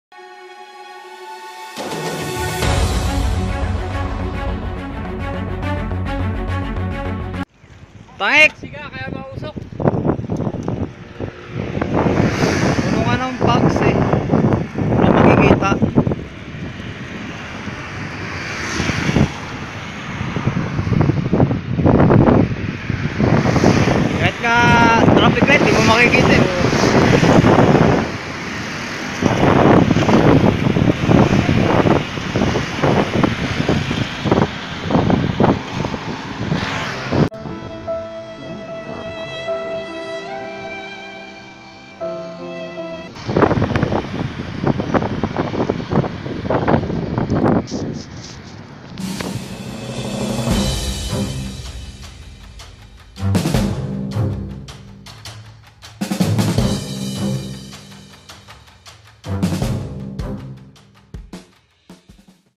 Moment sa pag uwi galing sa work.😂😜
Kaway kaway sa mga nasa Taiwan, oh kahit saan paman na lugar, nakakaranas ng malamig na panahon.😊ingatan nawa po😊